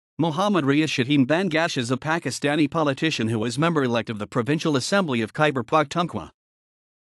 Muhammad Riaz Shaheen
Muhammad Riaz Shaheen Bangash is a Pakistani politician who is member-elect of the Provincial Assembly of Khyber Pakhtunkhwa.